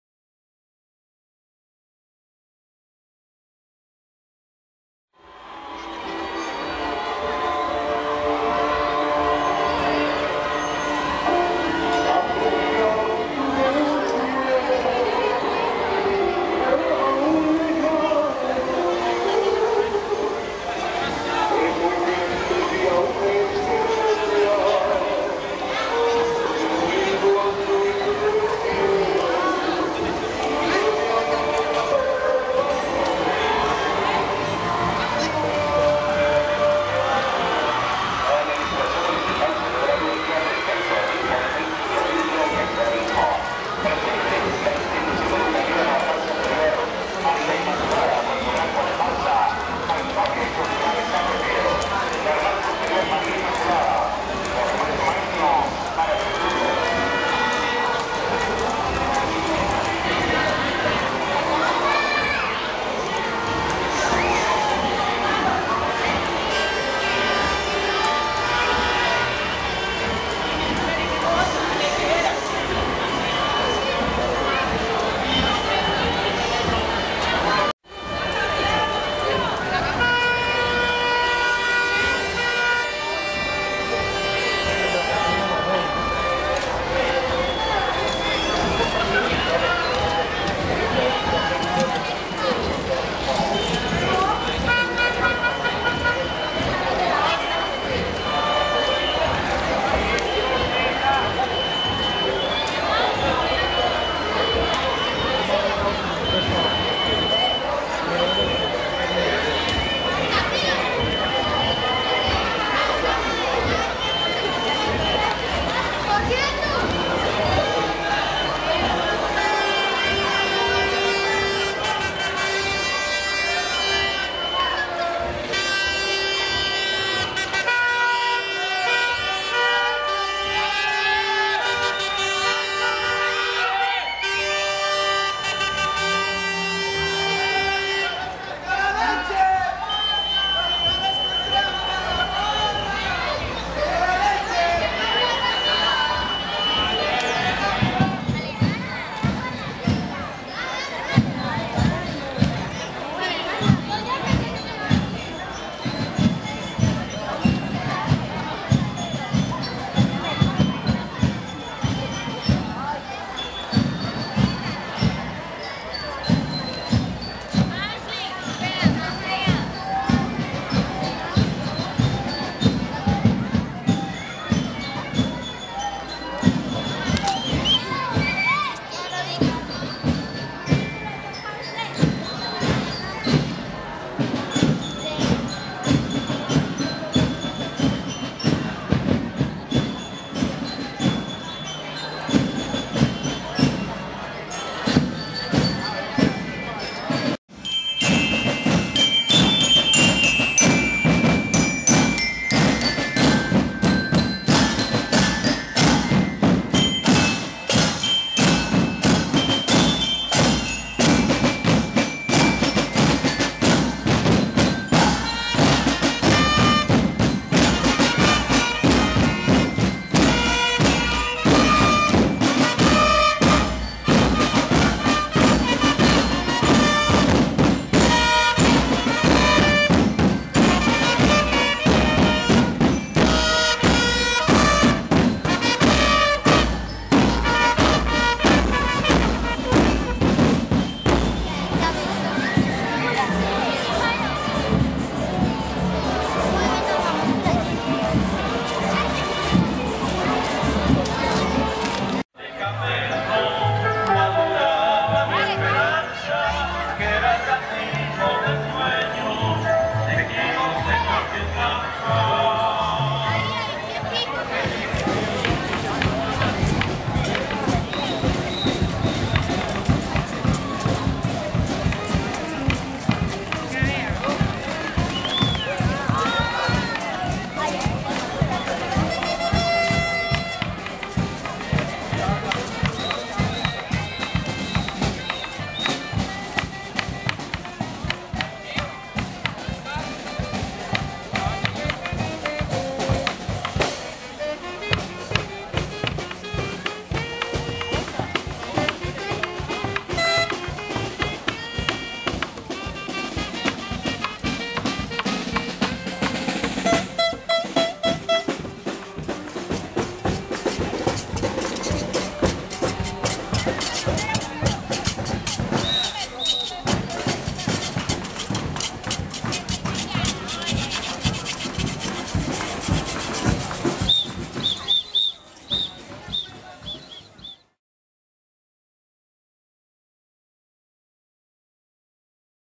Marcha Caicedonismo
Marcha de la Fraternidad y el amor por Caicedonia, realizada por todos los habitantes de Caicedonia en el día del Caicedonismo.